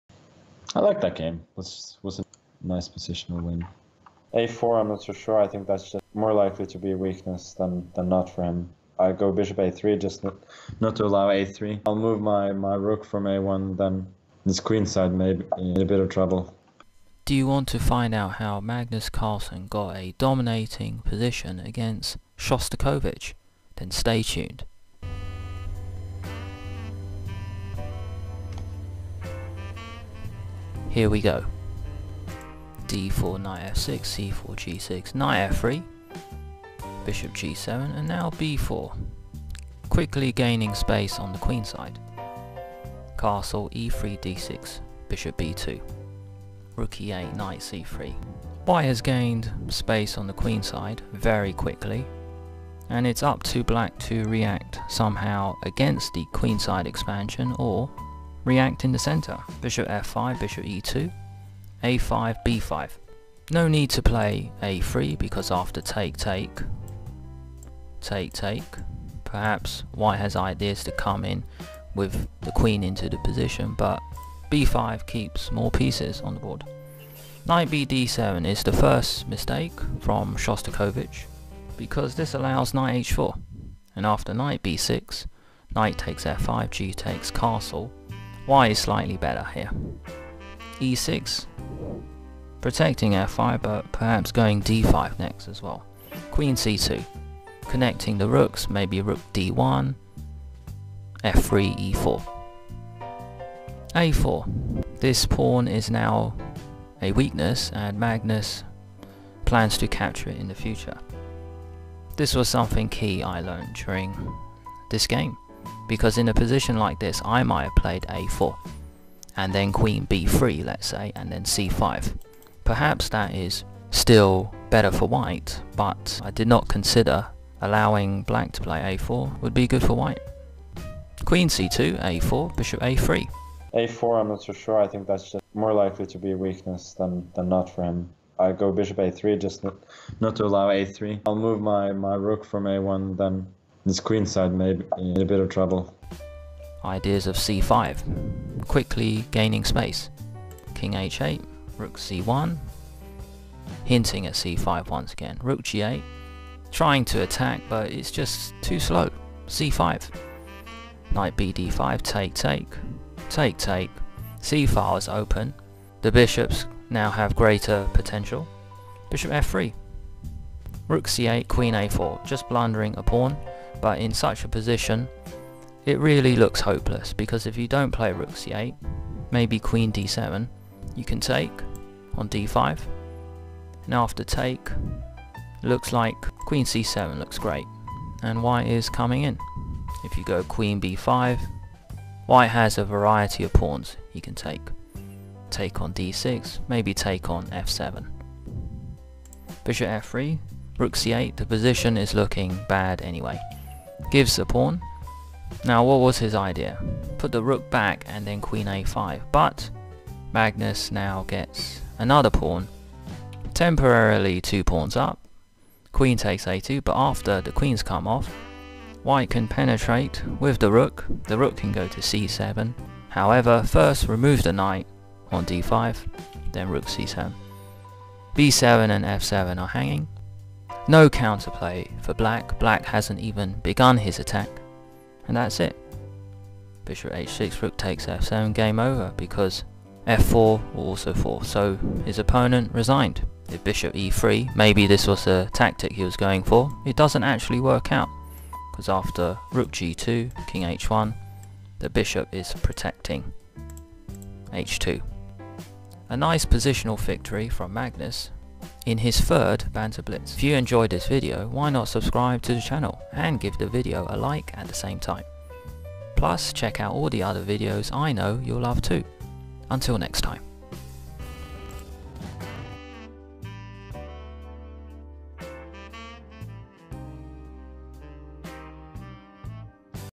Magnus Carlsen Beats Shostakovich
Magnus Carlsen Beats Shostakovich "I like that game. It was a nice positional win"
Magnus Carlsen beat chess 24 user Shostakovich in the 3rd banter blitz. In my opinion, this was one of his best games during his 3rd banter blitz.

What do you think of the game? Was it a nice positional win?
What do you think of this idea of playing b4 so early?
Please, turn on the bell to receive notification when I upload a new video